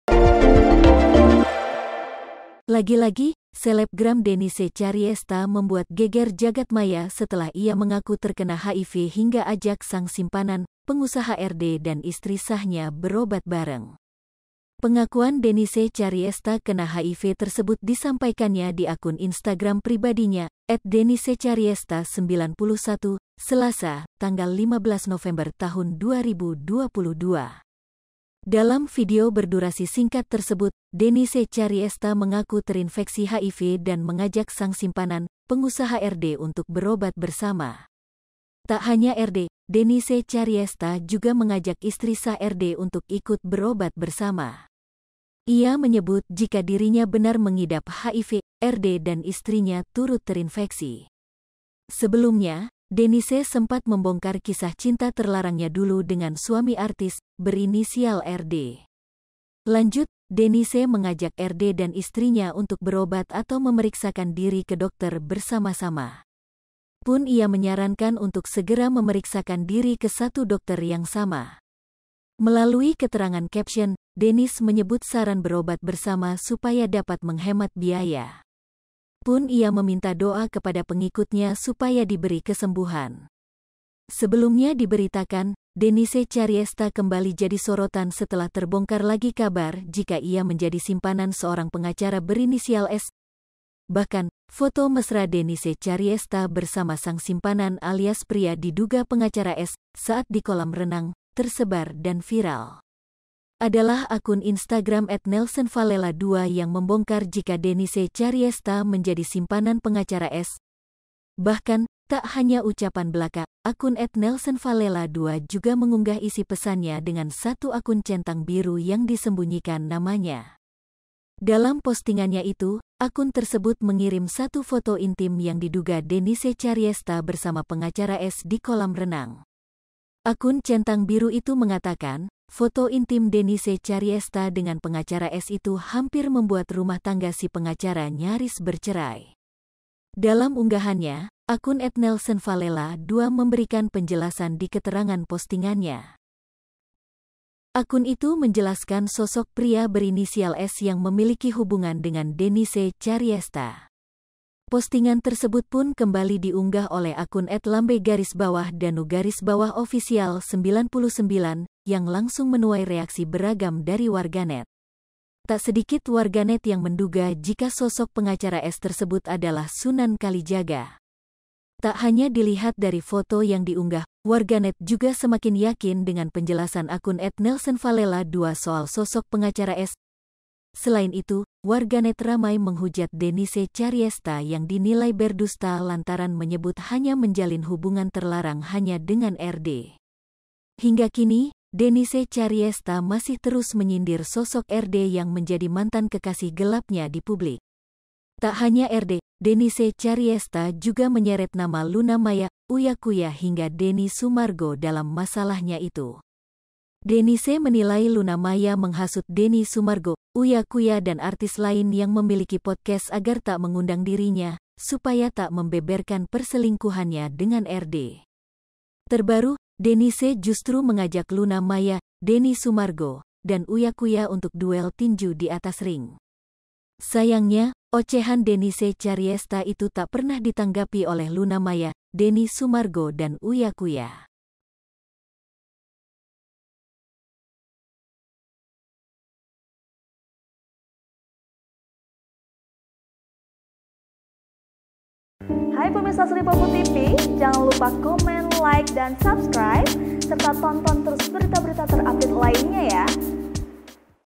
Denise Chariesta Mendadak Ngaku Kena HIV, Ajak RD dan Istrinya Berobat Bareng
Lagi-lagi, selebgram Denise Chariesta membuat geger jagat maya setelah ia mengaku terkena HIV hingga ajak sang simpanan, pengusaha RD dan istri sahnya berobat bareng.

Pengakuan Denise Chariesta kena HIV tersebut disampaikannya di akun Instagram pribadinya, @denisechariesta91, Selasa (15/11/2022).

Dalam video berdurasi singkat tersebut, Denise Chariesta mengaku terinfeksi HIV dan mengajak sang simpanan, pengusaha RD untuk berobat bersama.

Lihat informasi terkini bersama kami, di:

Palembang.tribunnews.com